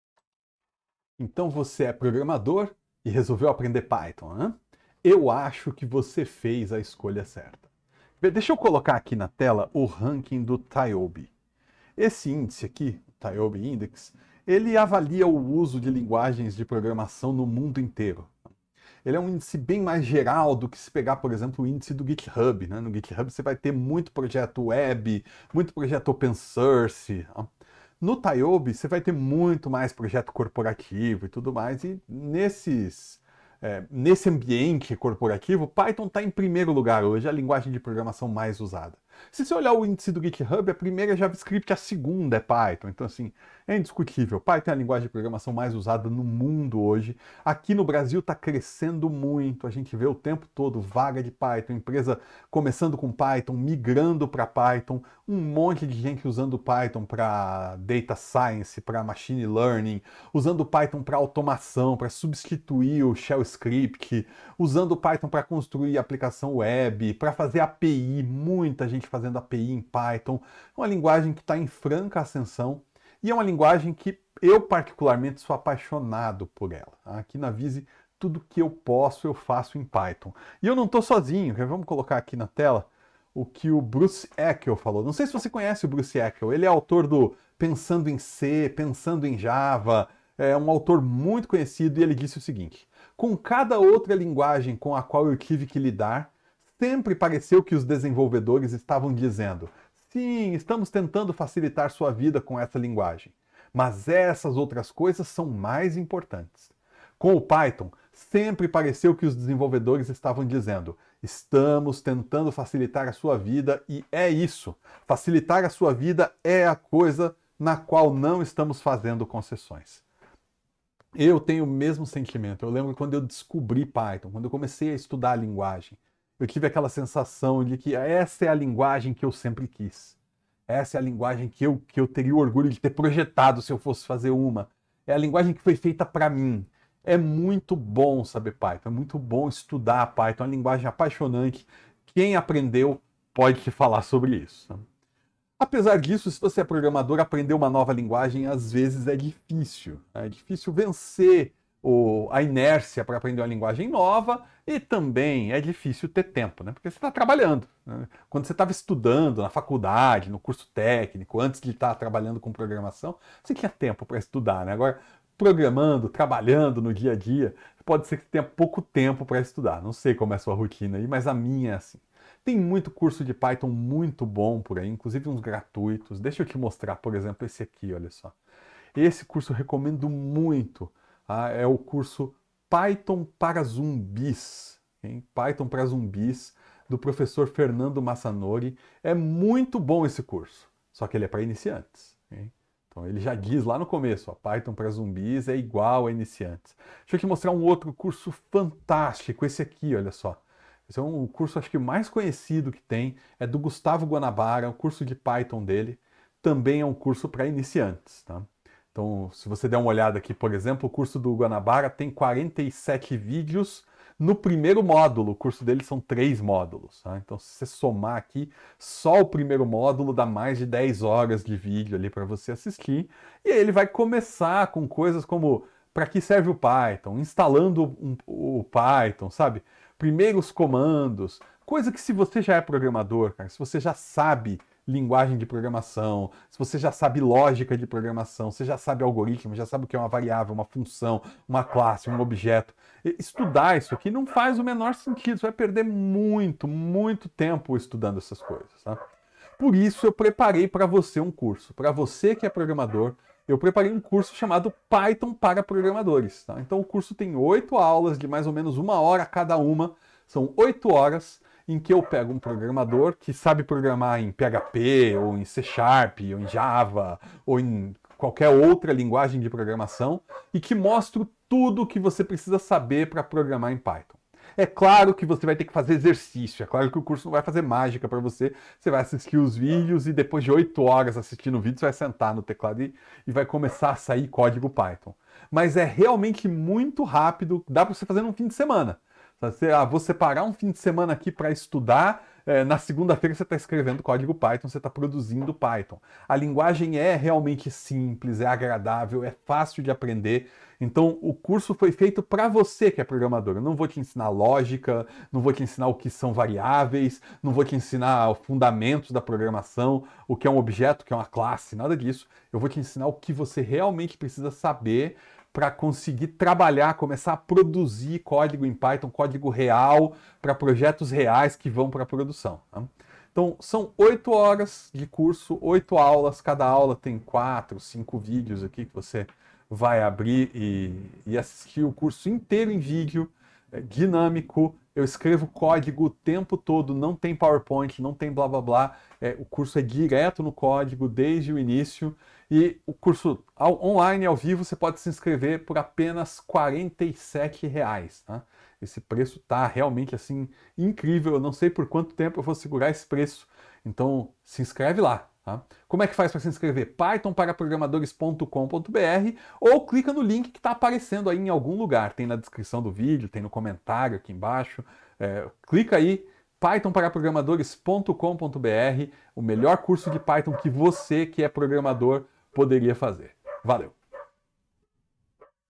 Python para Programadores
Você é um programador experiente. Já trabalha há algum tempo, talvez há anos, com linguagens como C#, PHP, JavaScript, Java, Ruby ou C++. Você resolveu aprender Python, mas os cursos que existem simplesmente não foram feitos para você. Longos, entediantes, foram feitos para iniciantes.

Esse curso é diferente! Em nove módulos de cerca de uma hora cada, vou mostrar a você tudo o que você precisa saber para mergulhar no Python e seu ecossistema. Não vou te ensinar lógica de programação, fundamentos da computação e todas as coisas que você já sabe. Você vai ver que, se já é programador, é muito fácil para você programar em Python e, em pouco tempo, se tornar realmente produtivo.

Curso em vídeo, 100% prático, sem Power Point ou blá blá blá, escrevo código o tempo todo, em todas as aulas. Ao final de cada aula há uma sessão de prática, em que faço um exercício demonstrando na prática como aplicar os conceitos daquela aula.

ATENÇÃO: Se você não é programador, não compre esse curso. Ele exige conhecimento dos fundamentos da computação e boa vivência em lógica de programação.